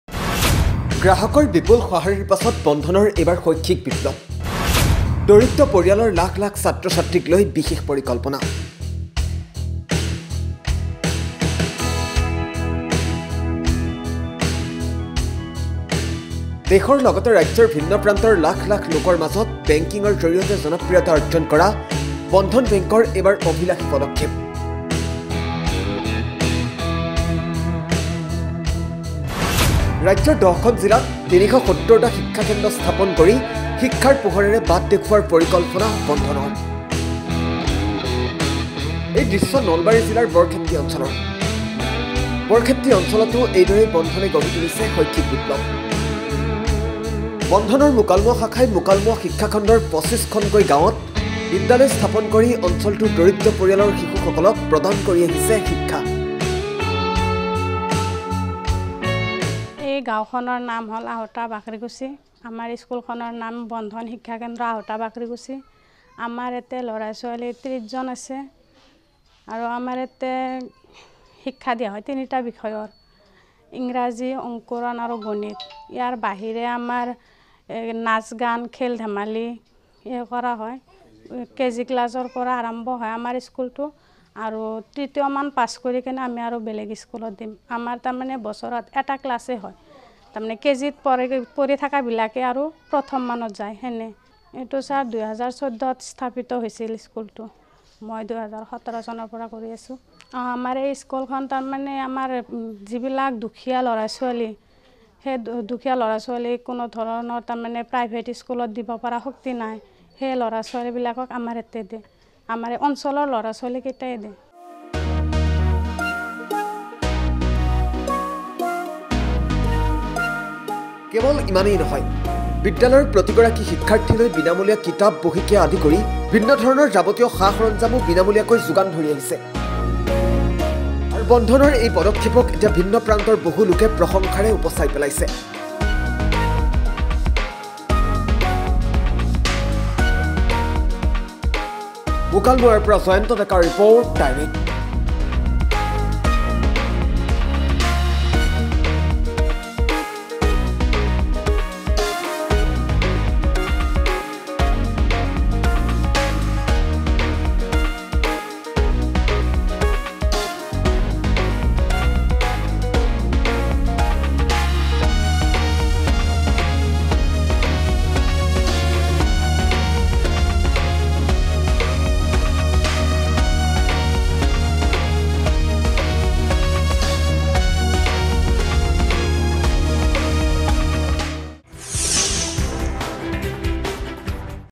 গ্ৰাহকৰ বিপুল সঁহাৰিৰ পিছত দৰিদ্ৰ পৰিয়ালৰ লাখ লাখ ছাত্ৰ-ছাত্ৰীক লৈ বন্ধন বেংকৰ বিশেষ পৰিকল্পনা
গ্ৰাহকৰ বিপুল সঁহাৰিৰ পিছত বন্ধন বেংকৰ শৈক্ষিক বিপ্লৱ। দৰিদ্ৰ পৰিয়ালৰ লাখ লাখ ছাত্ৰ-ছাত্ৰীক লৈ বিশেষ পৰিকল্পনা